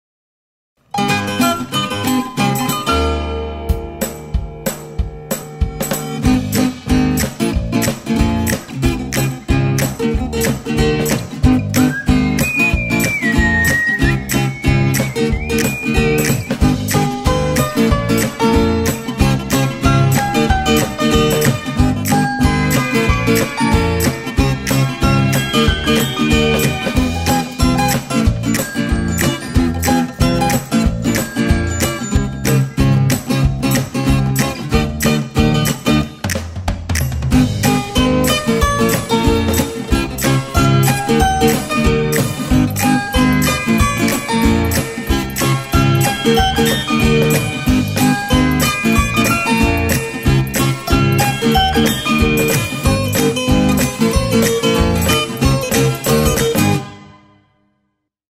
Torrada de Bolo de Milho São Braz
Torrada de Bolo de Milho. É isso mesmo! Preparamos uma receita incrível para você fazer e se deliciar. 💙